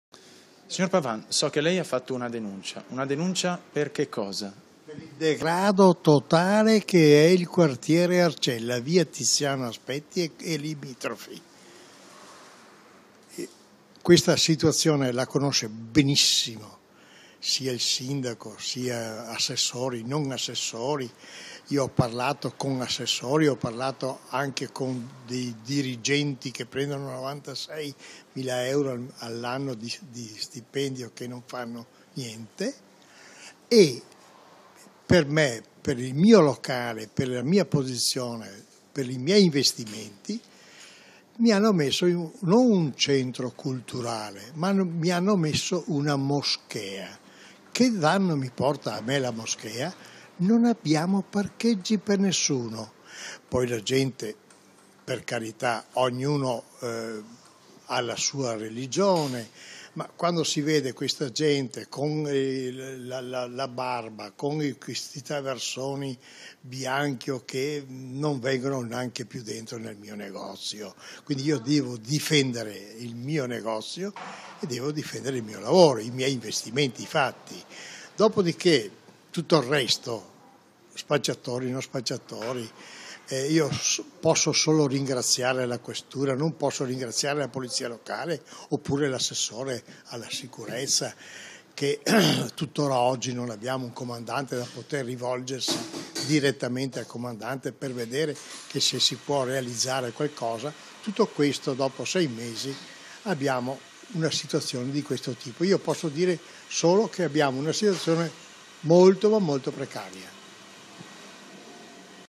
DEGRADO NEL QUARTIERE ARCELLA
Ermes Pavan titolare della gelateria "Tutto Gelato" in zona Arcella (pd) ha recentemente denunciato a gli organi prepost... [continua a leggere su